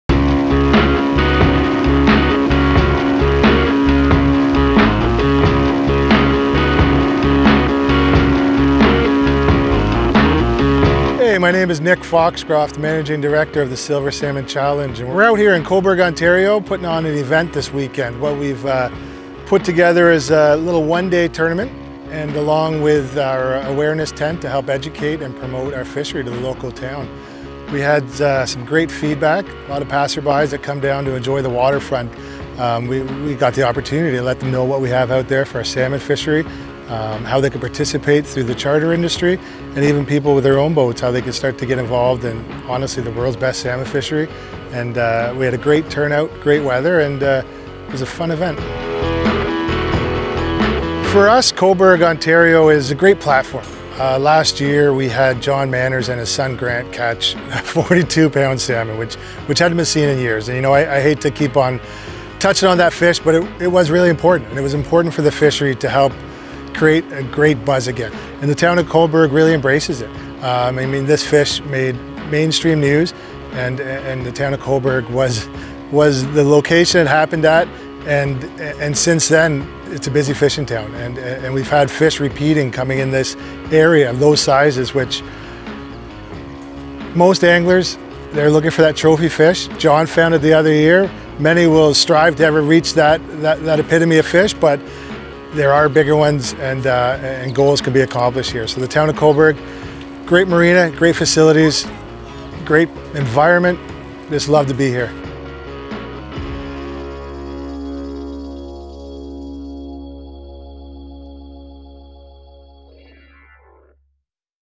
Cobourg Single Day Lake Wide Tournament
It was a great day in Cobourg Ontario, talking to people about Lake Ontario's incredible salmon fishing opportunities. Congratulations to the single day tournament winners.